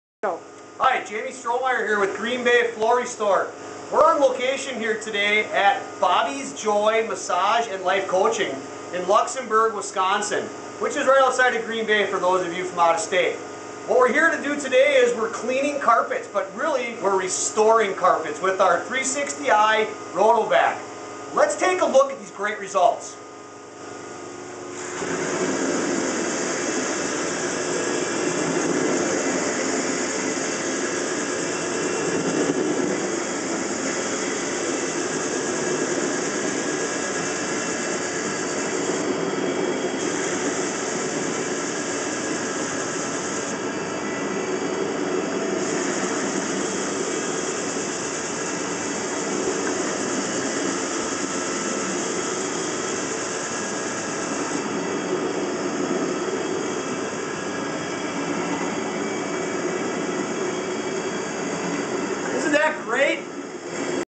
Carpet Cleaning Green Bay WI & Luxemburg WI Restorative Style.
Carpet Cleaning "restoratvie style" Green Bay WI & Luxemburg WI @ Bobbie's Joy Massage and Life Coaching